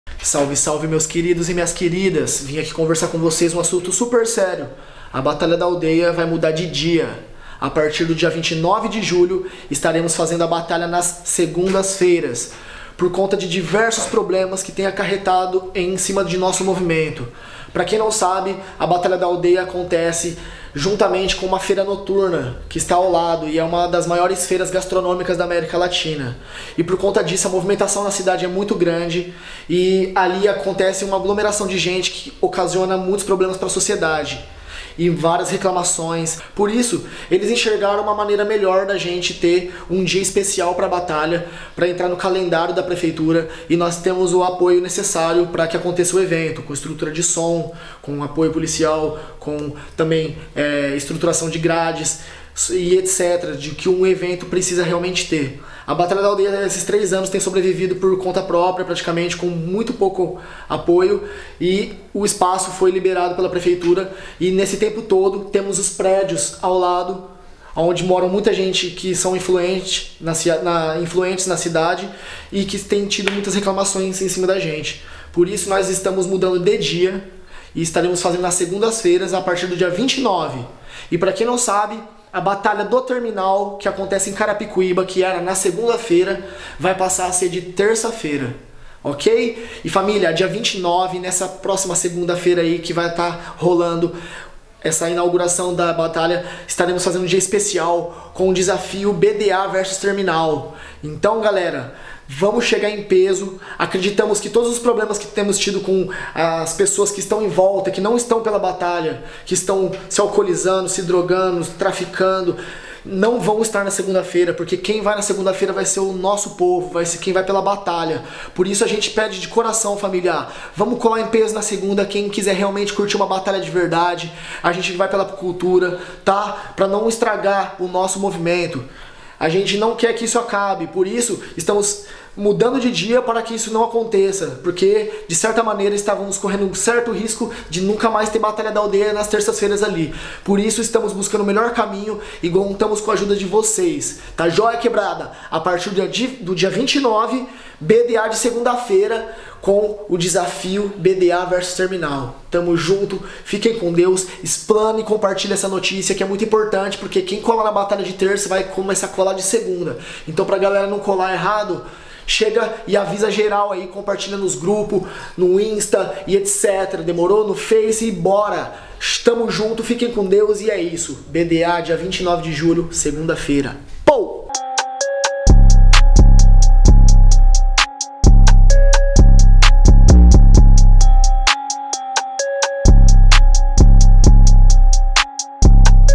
A BATALHA VAI MUDAR DE DIA? | NOVIDADES NA BDA
A partir do dia 29 de julho de 2019 a BATALHA DA ALDEIA será realizada todas as SEGUNDAS FEIRAS.

A mudança de dia consiste em uma melhoria para o ambiente que necessita cada vez mais de pessoas engajadas e pelo movimento HIP HOP.

Contamos com a ajuda de todos para comunicar as pessoas sobre a mudança para SEGUNDA FEIRA.

A Batalha continua no mesmo local e vai oferecer estrutura de banheiros químicos, barracas de alimentação, entre outras melhorias.

Nossa grade de conteúdo vai mudar somente o dia de postagem das Batalhas, que a partir de agosto vão ser as terças e quartas.

Esperamos que entendam a mudança e os motivos maiores.

A Equipe da Batalha da Aldeia agradece pela compreensão.